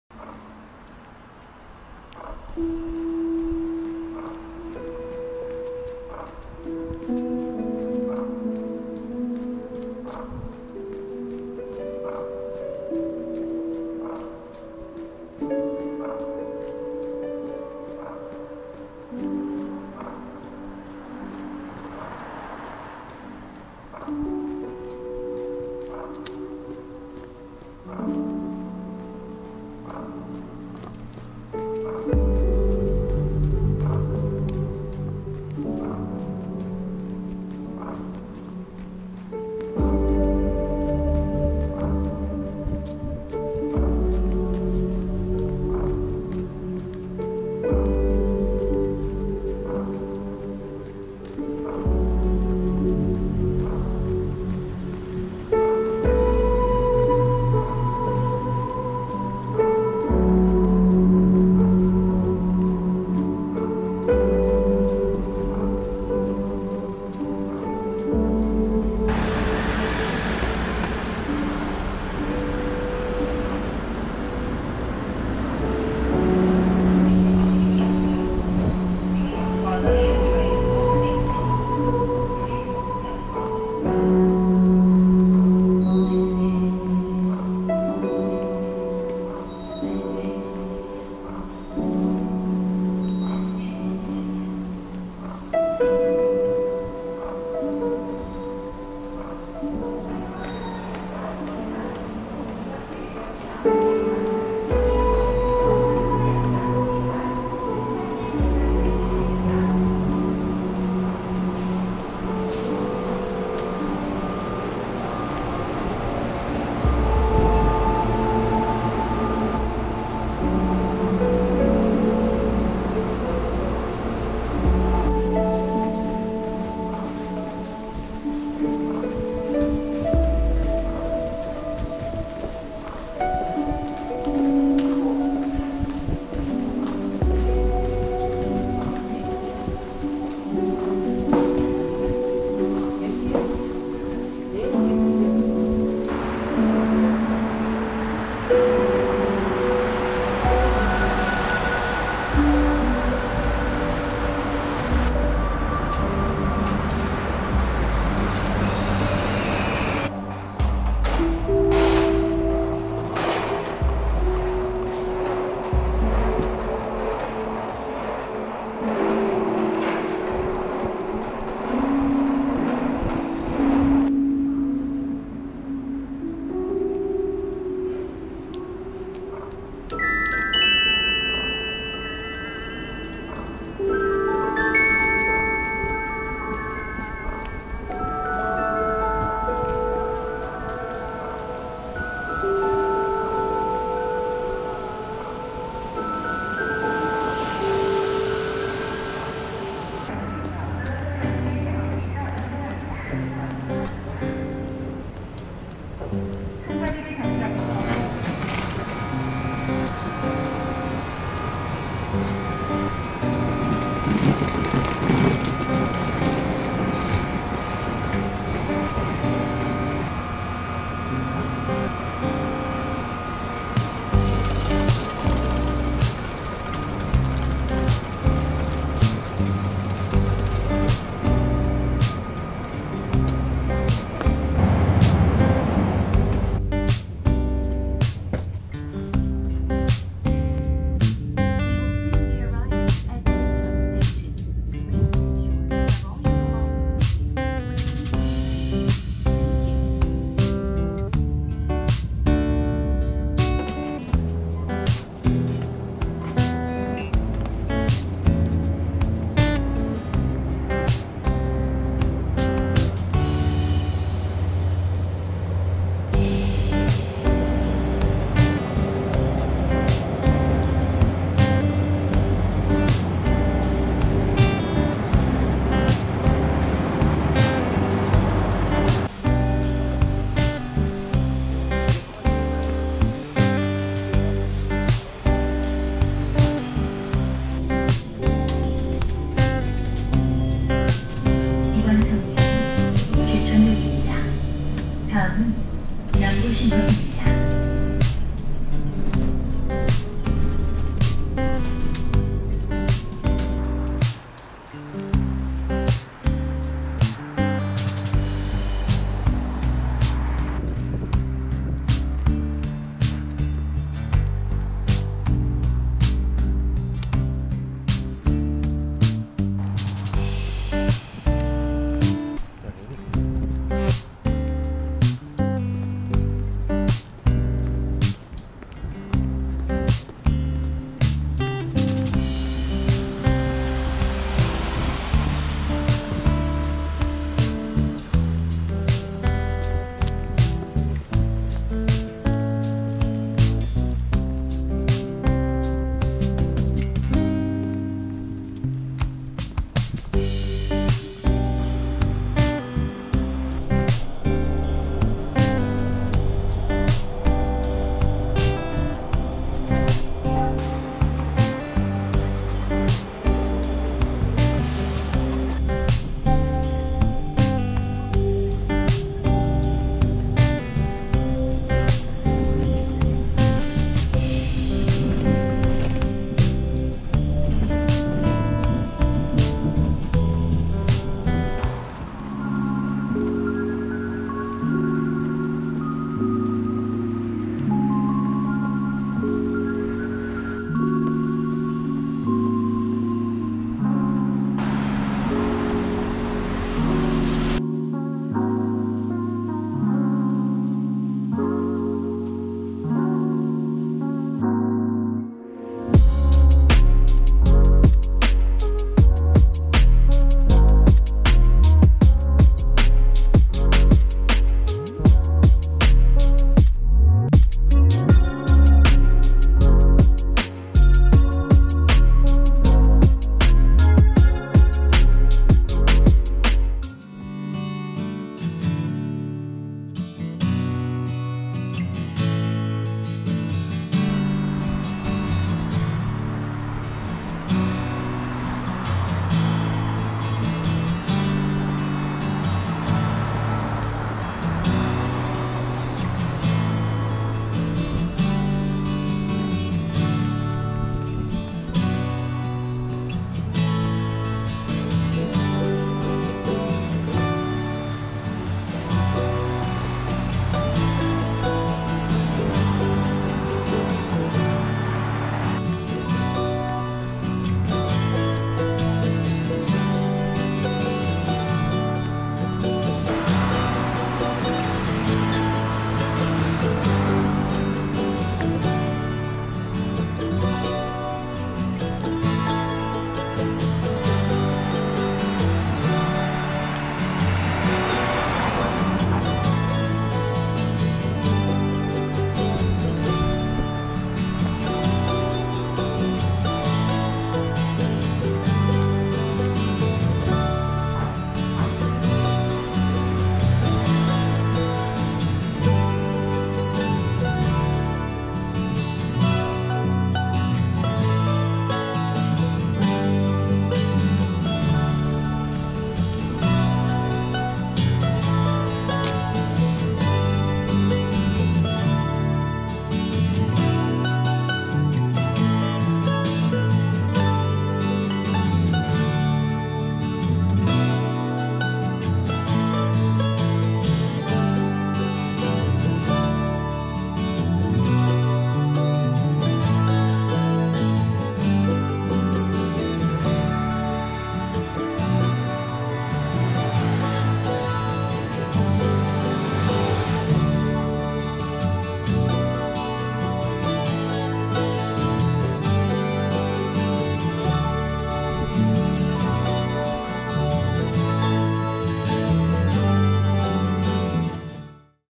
제천 하소동으로 가면 생기는 일｜아준 이야기
20211224

00:00 프롤로그 & 공지
01:09 화곡역 사거리
01:17 화곡역
02:05 신길역
02:40 청량리역
04:00 제천시청
04:13 제천역
05:03 의림지
06:00 제천시민회관 교차로
06:22 하소동
06:57 아점컴퍼니
(브런치 카페)
08:29 카페 지구대
(구 용두파출소, 2021년 1월 12일 오픈)

작년 12월 24일에 새벽 첫차를 탔습니다.
제가 제천 하소동으로 갔는데, 다 같이 걸어볼까요.